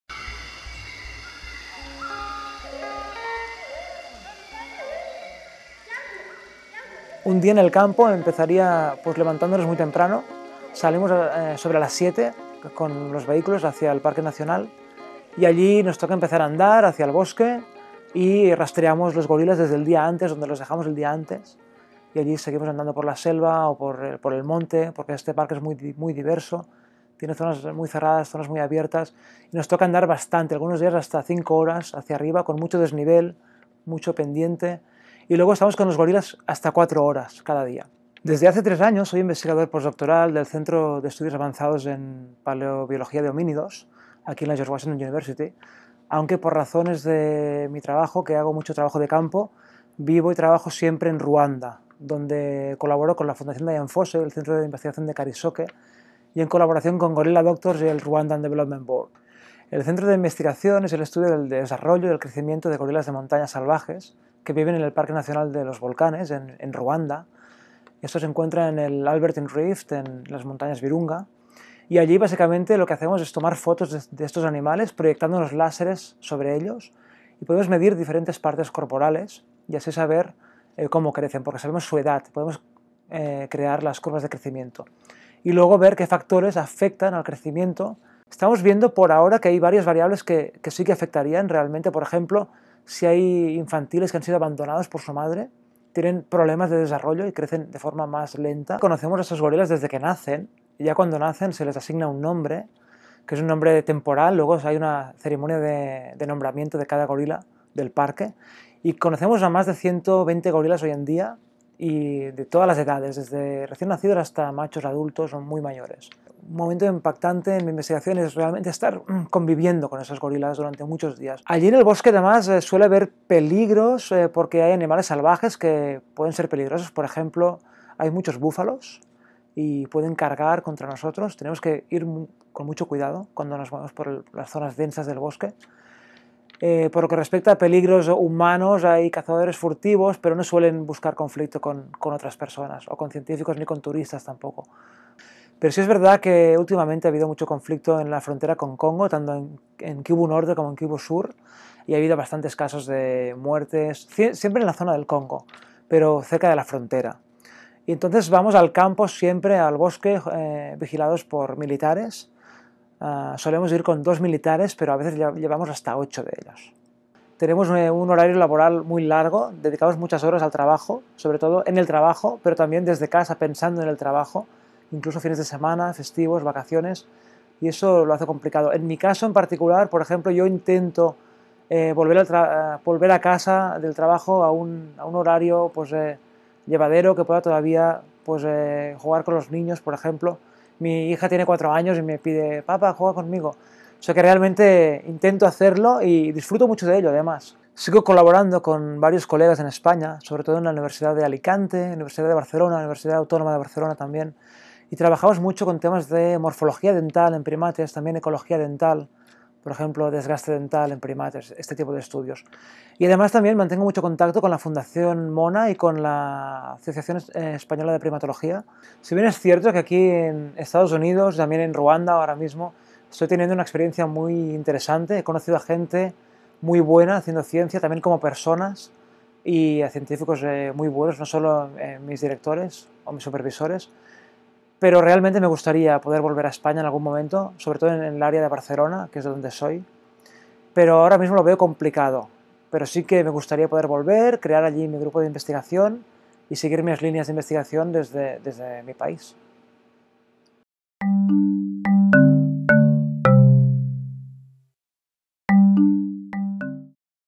Jordi Galbany: Among Gorillas, United States to Rwanda
ECUSA presents EVisibilityTV from the heart of the Virunga Mountains in Rwanda; the first of a series. Today, we join Jordi Galbany, as he takes us deep into the forest to give us an intimate glimpse at the lives of the Mountain Gorilla. Tune in every Thursday for more exciting Spanish scientist at work!

Subtitles available in English/ Subtítulos disponibles en español.

Follow us in and the social networks!